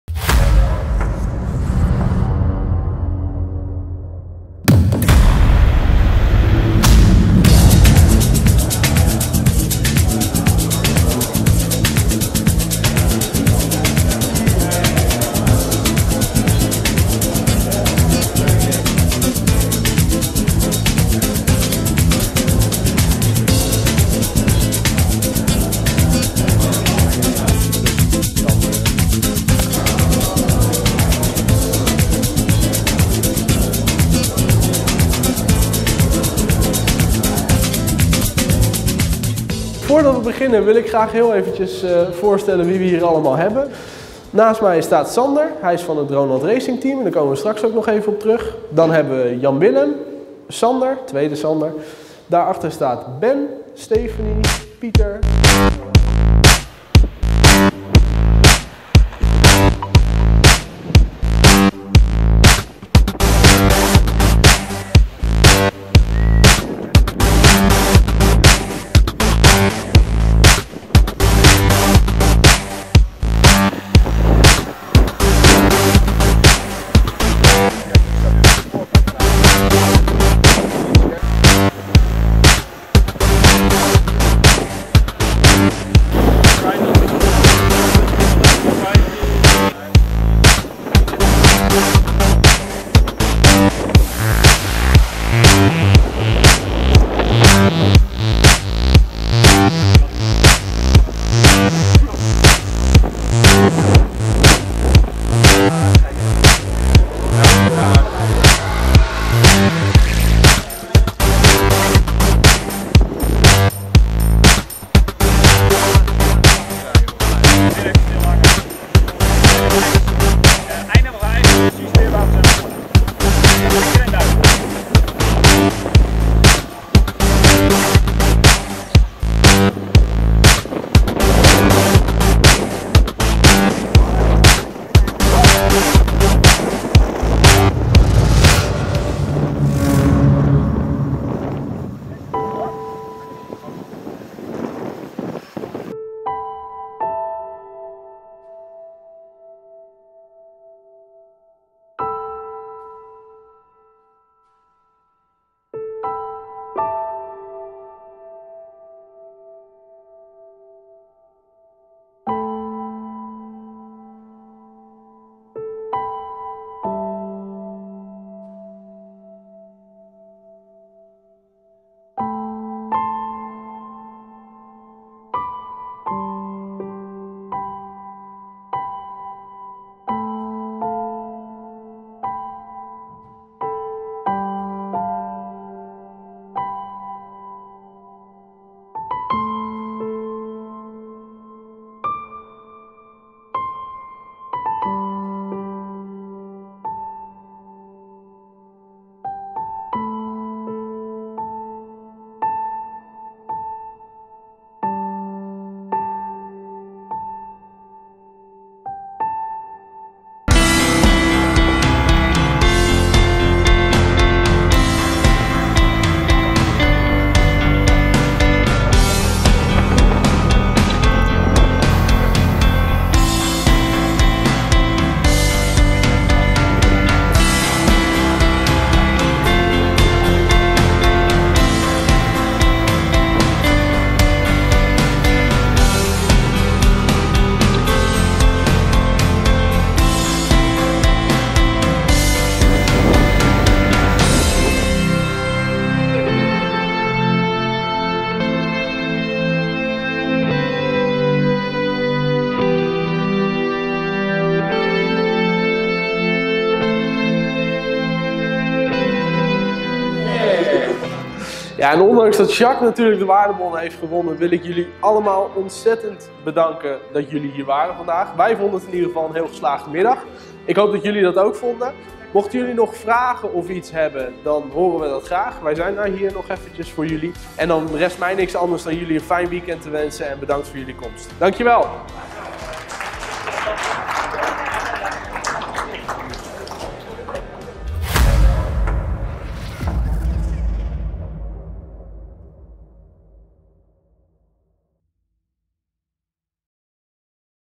DJI & DroneLand: New Pilot Experience 18-06-2016
Op zaterdag 18 juni 2016 organiseerde wij weer een New Pilot Experience! Tijdens dit evenement kunnen mensen die nog nooit hebben gevlogen een korte theoretische uitleg krijgen, om vervolgens onder begeleiding van onze instructeurs veilig hun eerste meters te vliegen. Al met al was het een zeer geslaagde dag, waarvan je hierbij een video impressie vindt. Iedereen bedankt voor je komst, en wellicht tot de volgende!